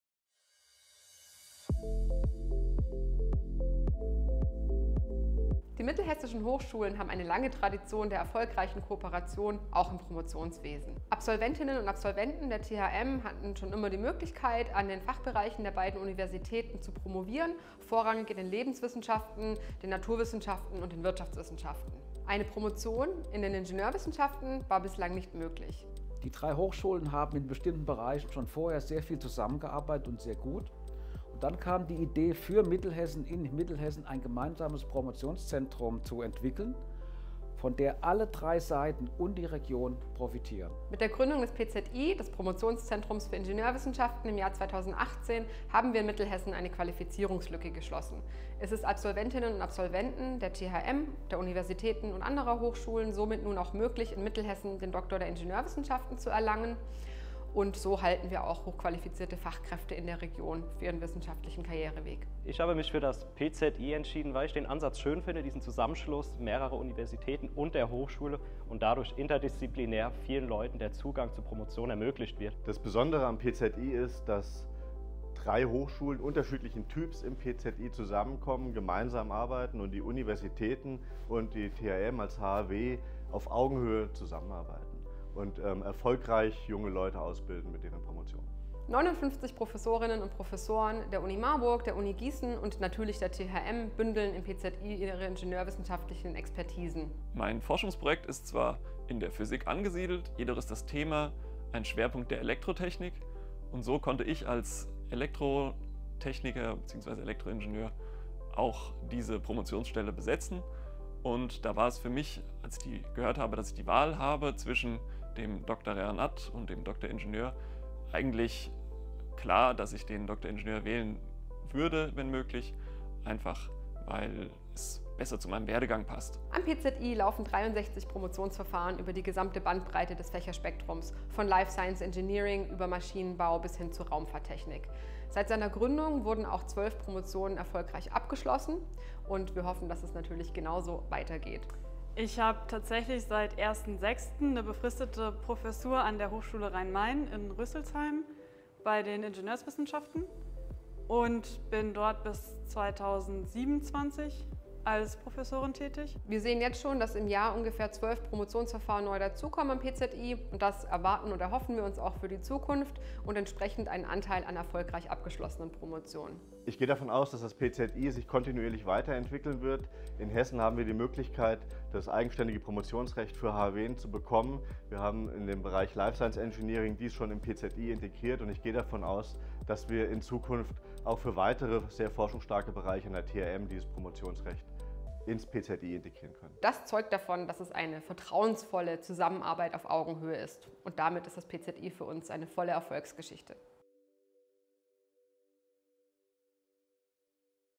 Das Promotionszentrum für Ingenieurwissenschaften (PZI)
Im Promotionszentrum für Ingenieurwissenschaften (PZI) am Forschungscampus Mittelhessen können Sie in folgenden Fächern promovieren:

- Agrar- und Umweltwissenschaften
- Bauingenieurwesen
- Biotechnologie und -pharmazeutik
- Elektrotechnik
- Informatik
- Life Science Engineering
- Maschinenbau
- Materialwissenschaften
- Medizin- und Gesundheitstechnik
- Raumfahrttechnik

Promotionen am PZI sind in der Regel kooperative Promotionen zwischen der Technischen Hochschule Mittelhessen (THM) und der Justus-Liebig-Universität-Gießen (JLU) bzw. der Philipps-Universität Marburg (UMR). Abgeschlossenen Promotionen liegt das universitäre Promotionsrecht der JLU oder der UMR zugrunde. Ausnahme ist der Bereich Life Science Engineering, für den die THM seit April 2019 das Promotionsrecht innehat.